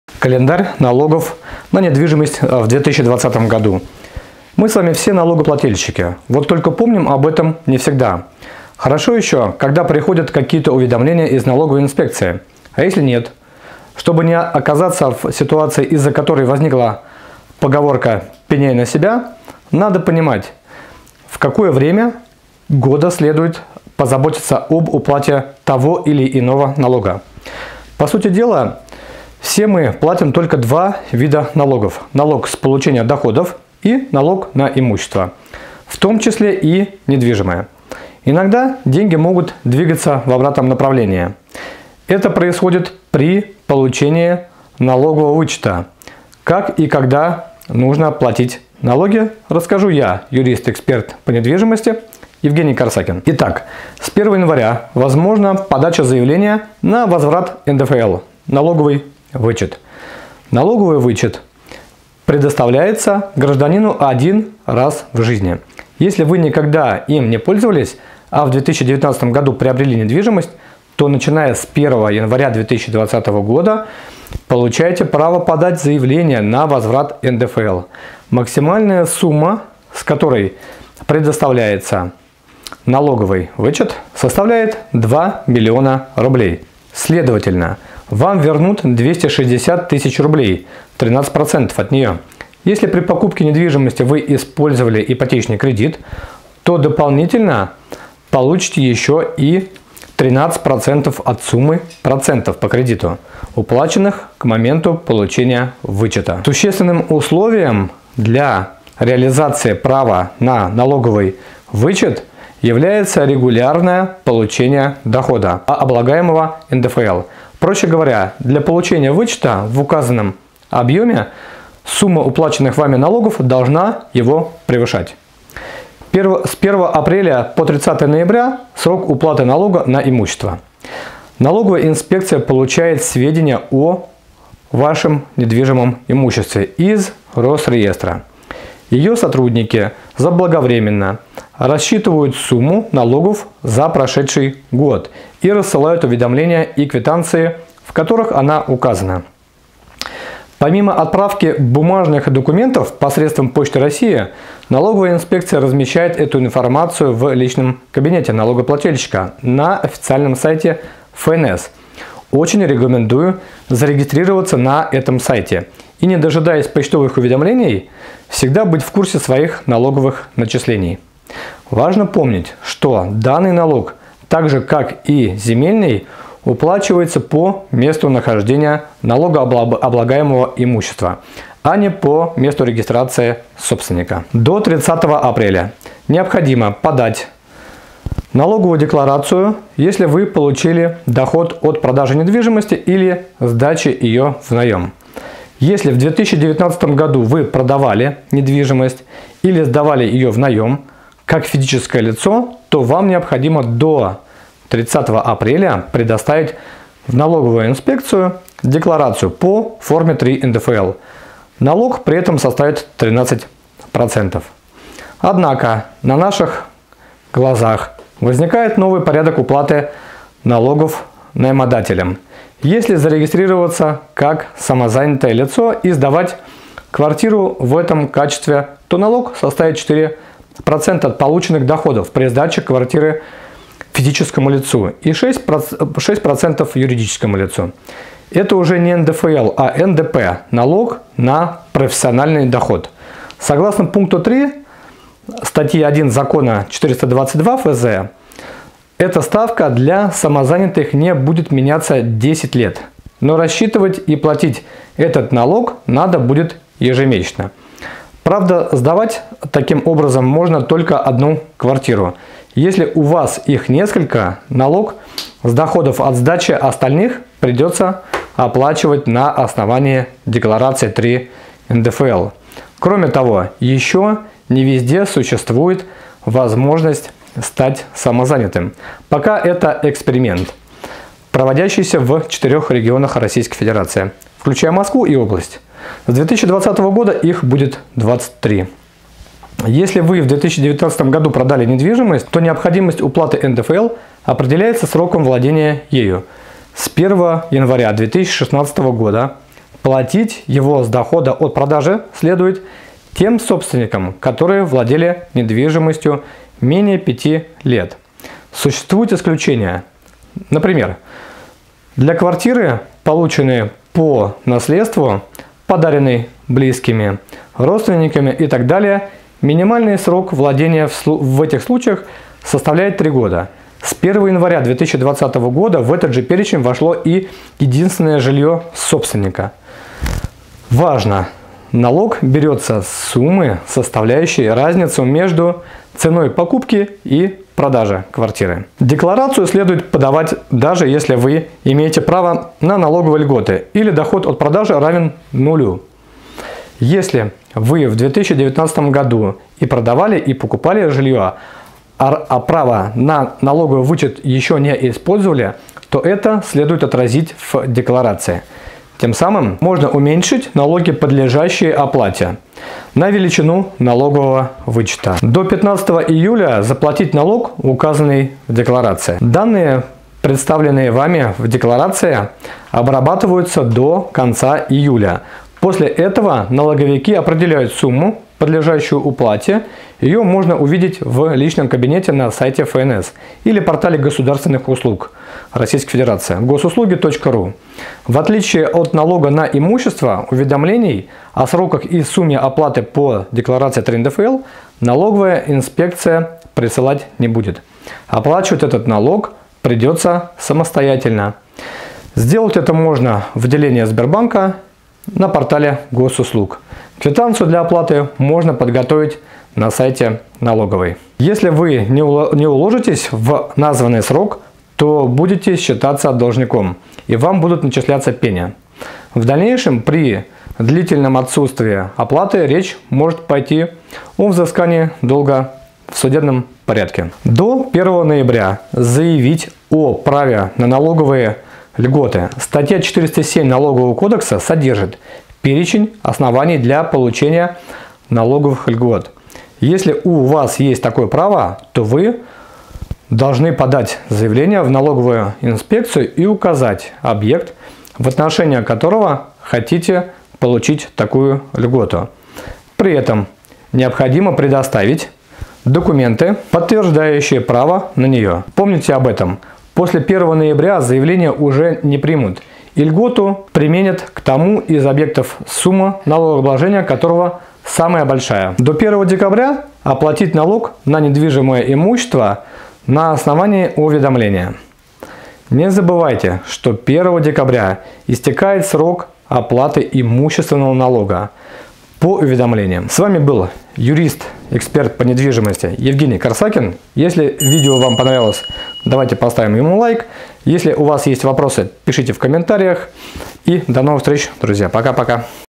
Календарь налогов на недвижимость в 2020 году.
в этом видео Вы узнаете:
👉 Какой  календарь налогов на недвижимость в 2020 году
►С 1 января: возможна подача заявления на возврат НДФЛ (налоговый вычет)
►1 апреля – 30 ноября: срок уплаты налога на имущество
►До 30 апреля: необходимо подать налоговую декларацию, если вы получали доход от продажи недвижимости или сдачи ее внаем. Если в 2019 году вы продавали недвижимость или сдавали ее внаем, как физическое лицо, то вам необходимо до 30 апреля предоставить в налоговую инспекцию декларацию по форме 3-НДФЛ. Налог при этом составит 13%.
►До 15 июля: заплатить налог, указанный в декларации
►До 1 ноября: заявить о праве на налоговые льготы
►До 1 декабря: оплатить налог на недвижимое имущество на основании уведомления.